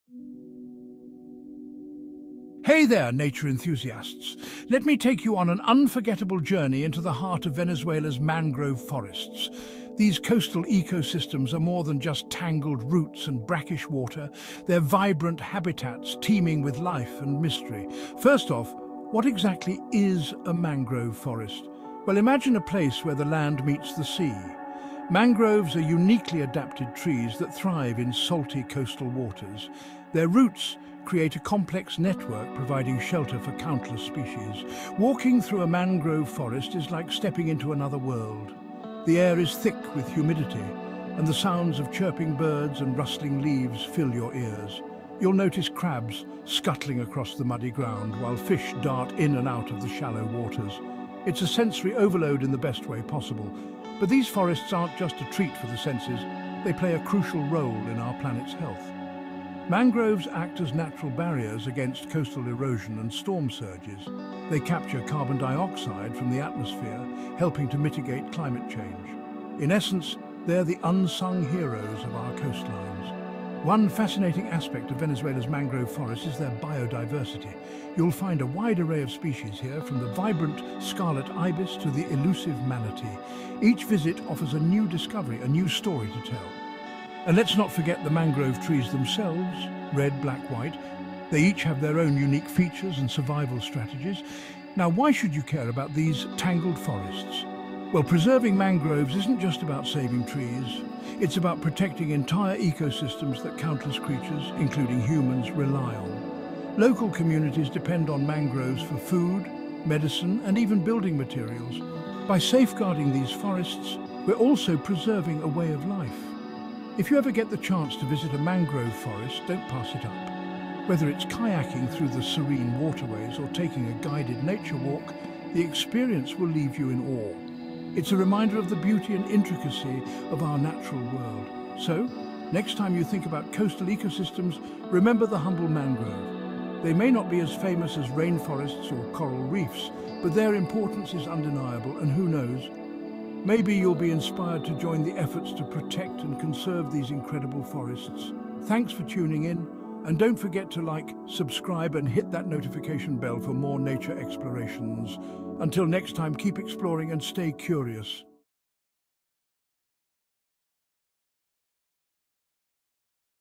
Mangrove Mysteries: Venezuela's Coastal Ecosystems Explored
Venture into the tangled mangrove forests lining Venezuela's coastlines and uncover the secrets of these vital coastal ecosystems. Wander through the labyrinth of salt-tolerant trees and their intertwining roots, which create a unique habitat crucial for numerous species. Observe how mangroves provide shelter and breeding grounds for fish, birds, and crustaceans, supporting a rich web of life. Discover their essential role in protecting coastal areas from erosion and storm surges, acting as natural barriers against the forces of the sea. Learn about the complex adaptations of these resilient trees, from their aerial roots that breathe in oxygen to their ability to filter salt from seawater. This exploration reveals the intricate balance of life within these wetlands and highlights the need for conservation to protect these invaluable ecosystems from threats such as climate change and habitat destruction. Join us to experience the wonder and importance of Venezuela’s mangrove forests.